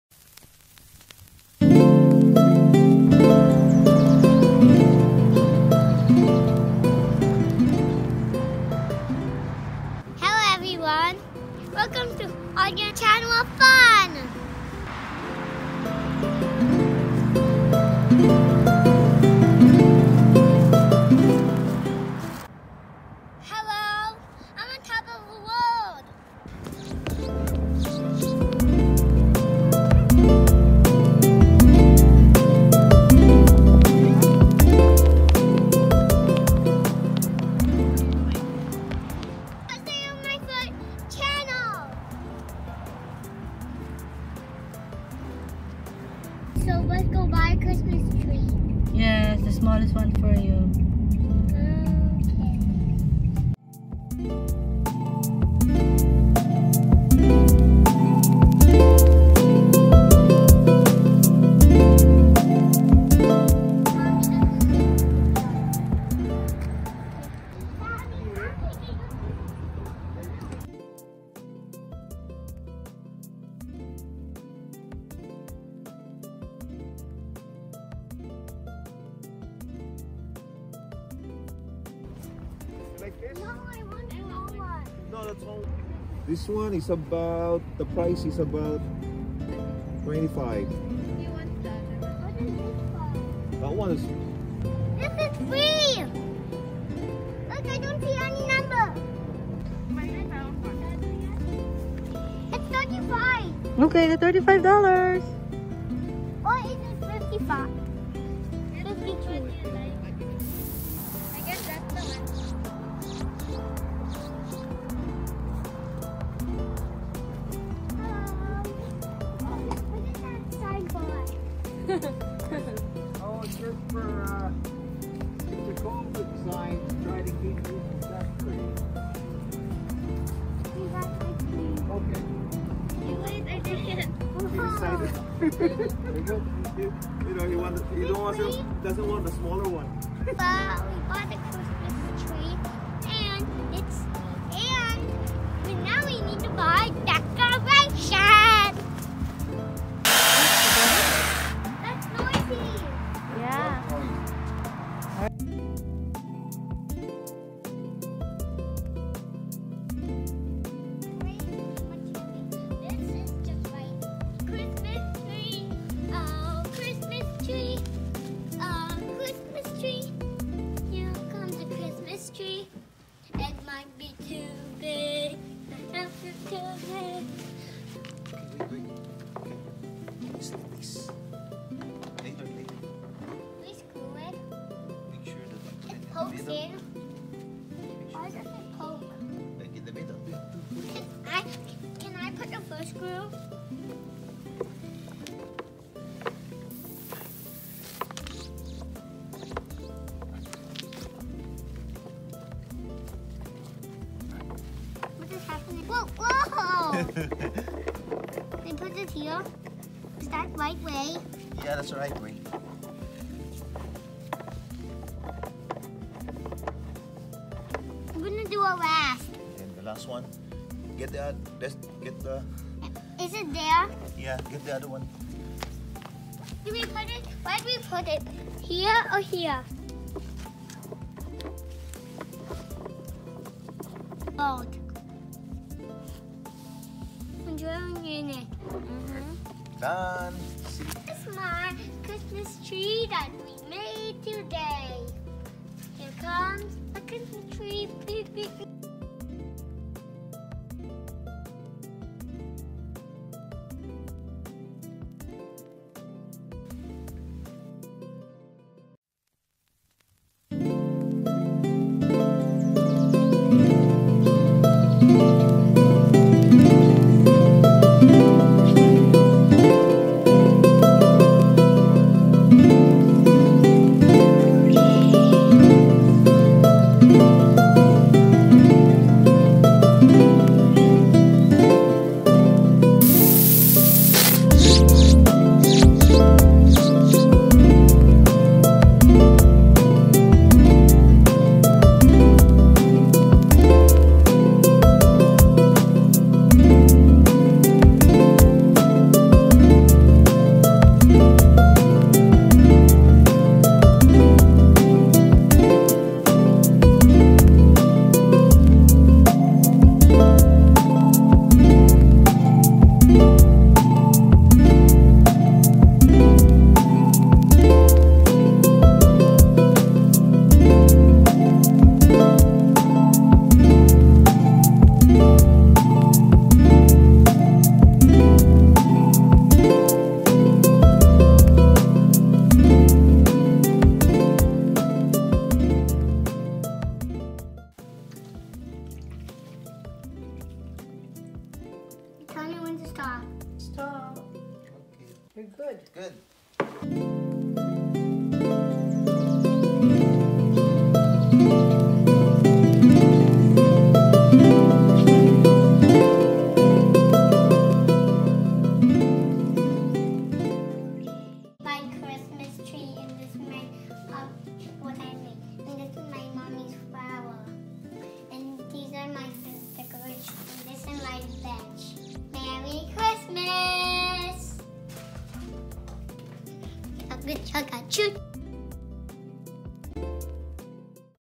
First Day of December: Picking out a Christmas Tree
Hello Everyone and Welcome to Ardrean Tans of FUN!!!

In this video, I, Ardrean, will be picking a Christmas Tree and decorating it with my FAMILY!!!

I'll see you soon on Ardrean Tans of FUN channel.